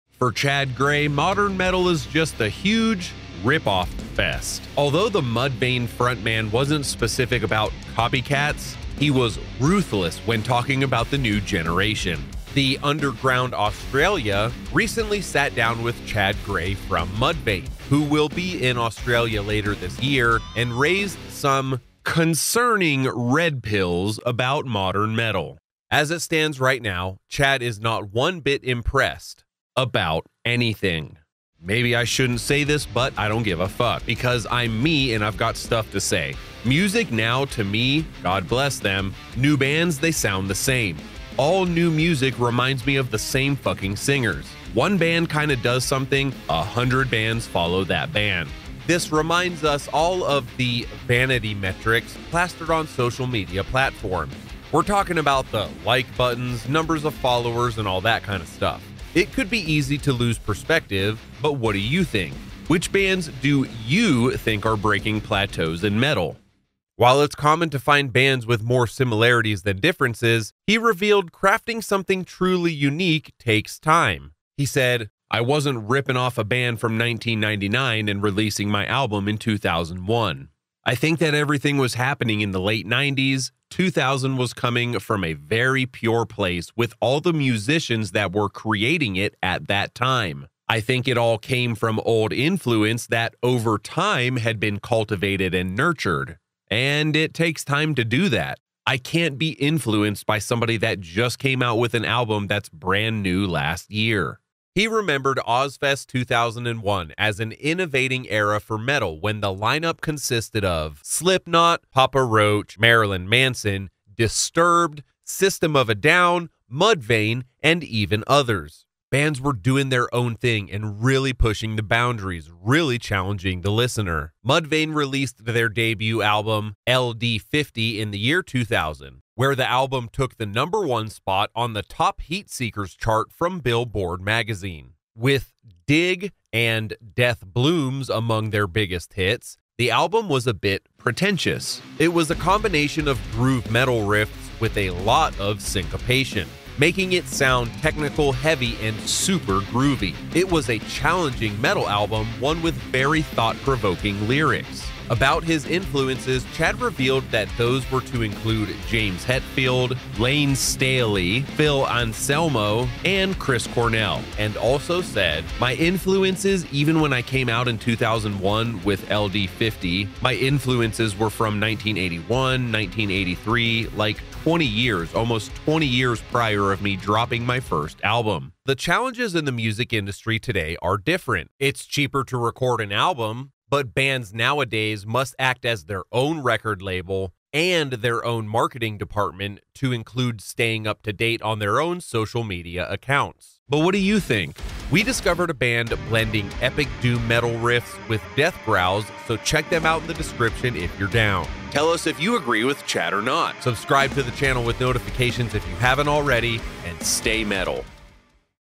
Chad Gray From Mudvayne Doesn't Dig Modern Metal
Though the Mudvayne frontman wasn't specific about the "copycats," he was ruthless. “The Underground Australia” recently sat down with Chad Gray and his thoughts are currently breaking the internet.